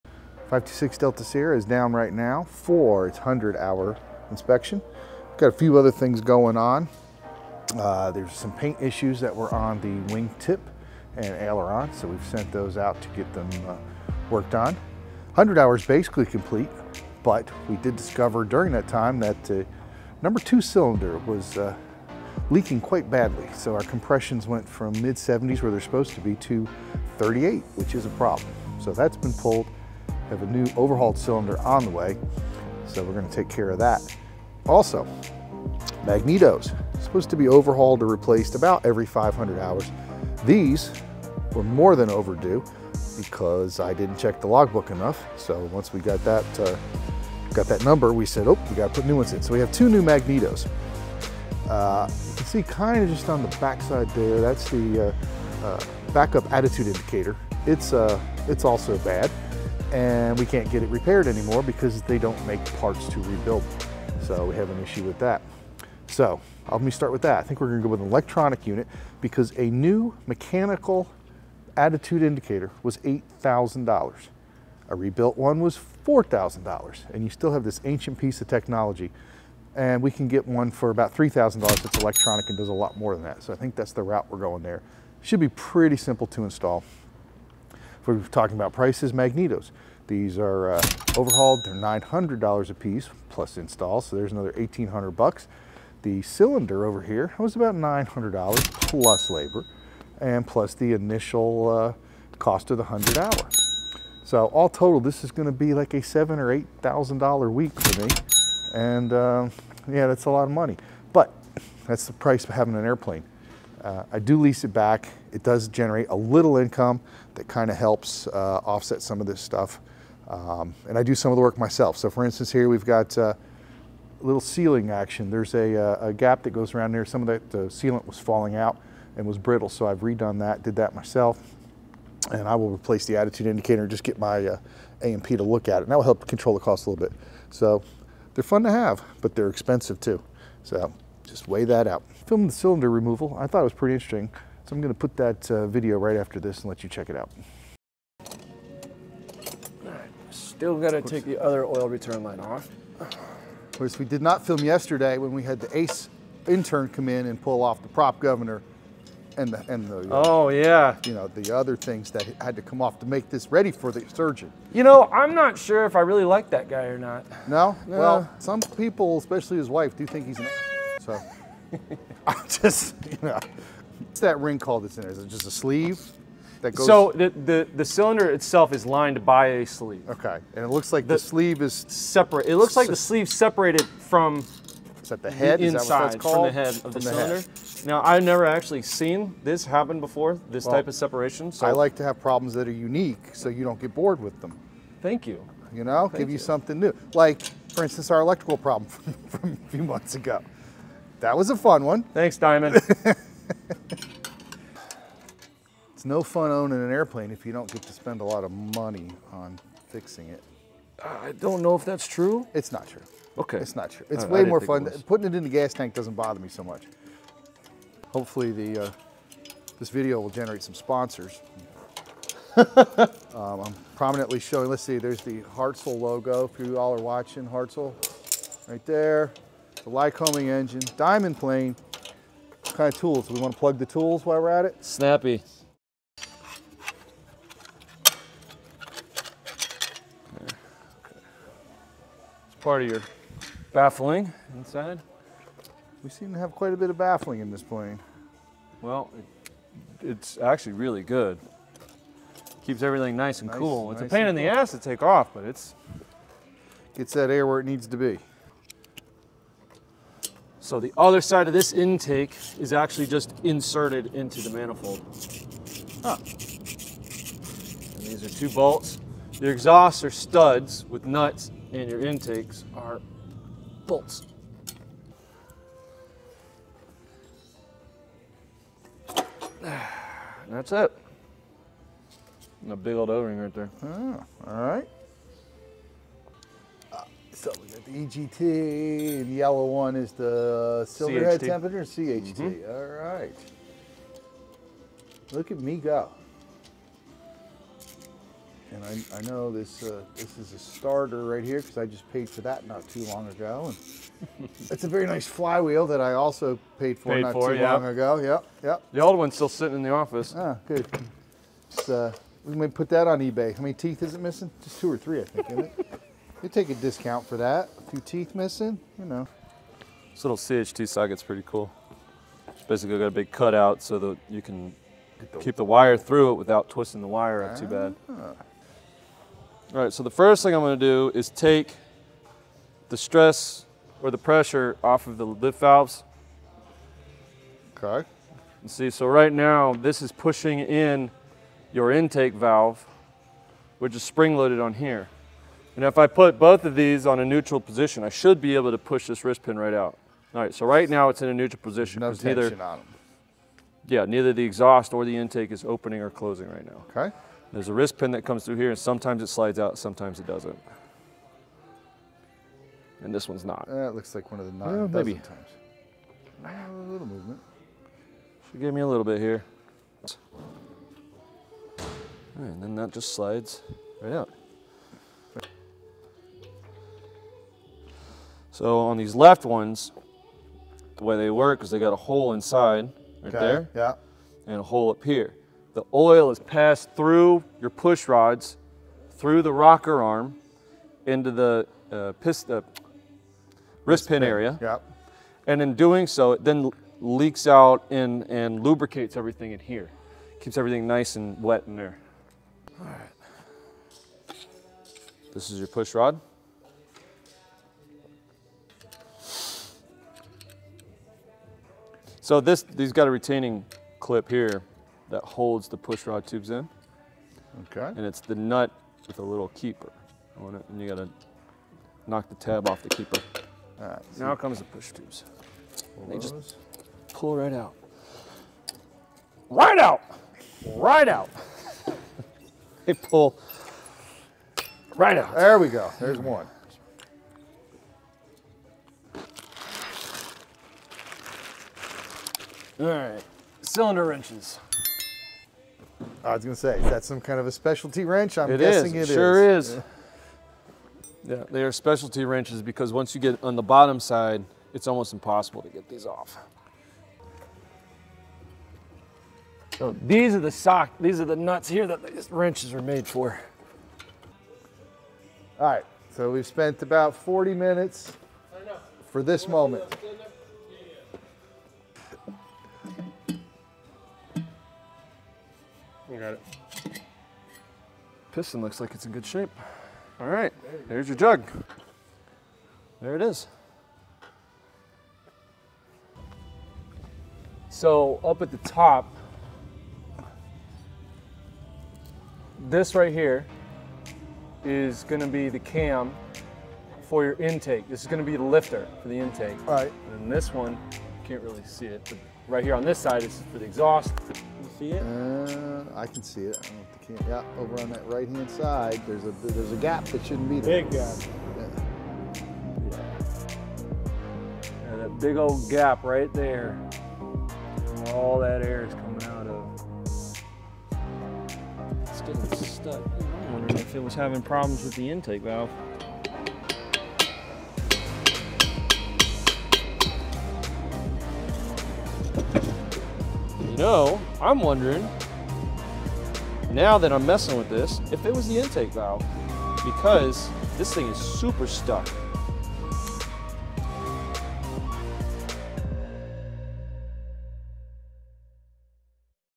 Airplane Cylinder Removal-Diamond DA40
Airplane cylinder removal (from a Diamond DA40) is not fun or cheap but, a compression test during the 100 hour inspection revealed a major loss of compression on cylinder #2. It needs to be removed for inspection.

00:00 Introduction
01:07 Cost Estimates
02:34 Engine Cylinder Removal
08:07 Looking For The Problem